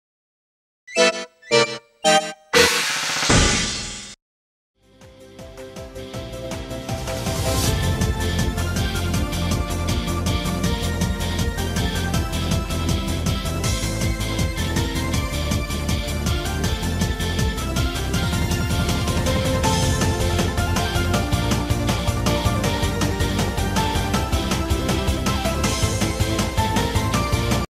New Channel Intro and Outro
we decided to remake the Intro and Outro.
with more details, and New colours and designs.